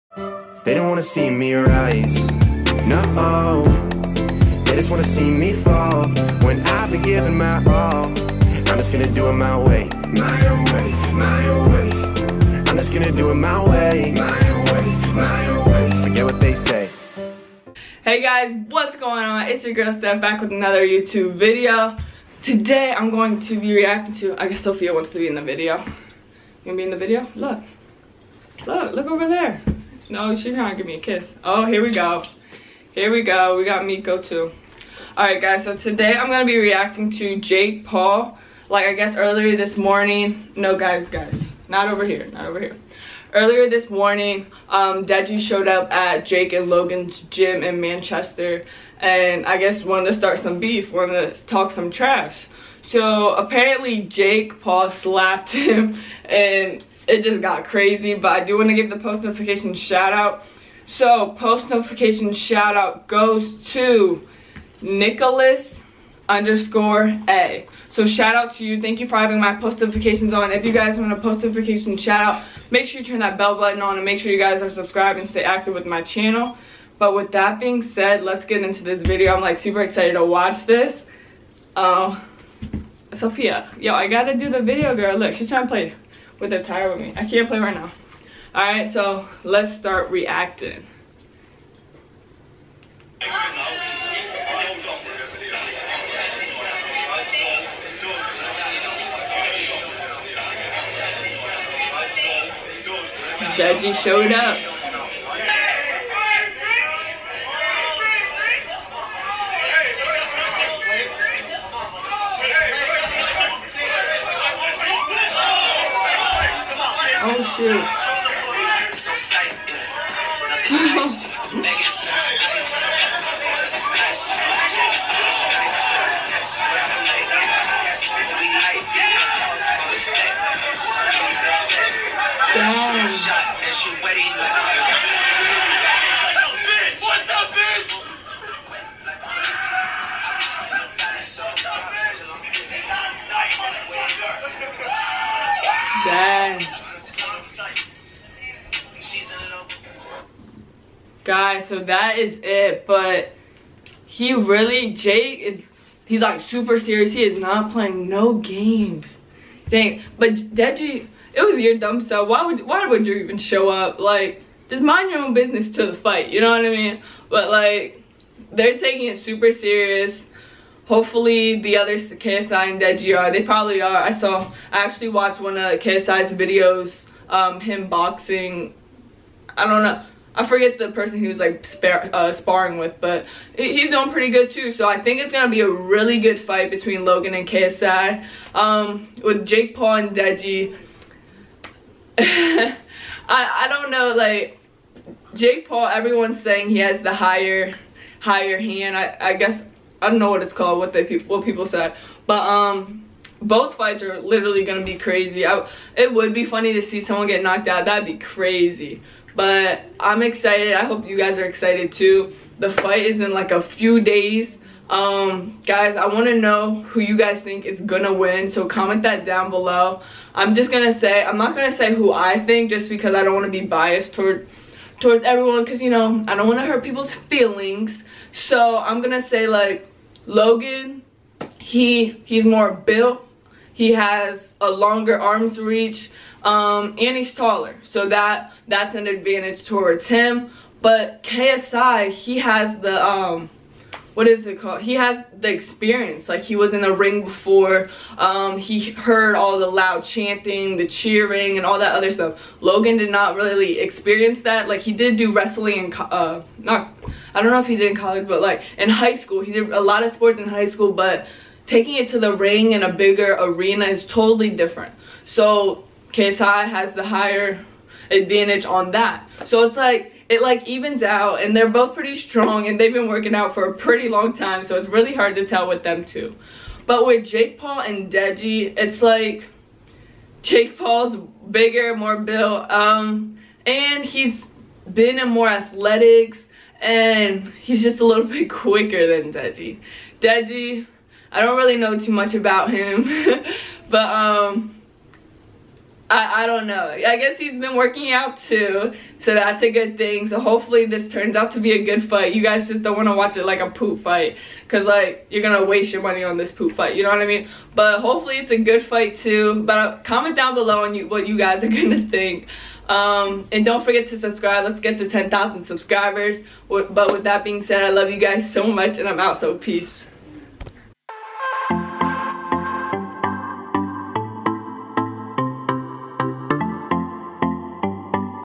JAKE PAUL SLAPPED DEJI IN THE FACE!! (Unseen Footage)
SNAPCHAT (@Stefaniekebede)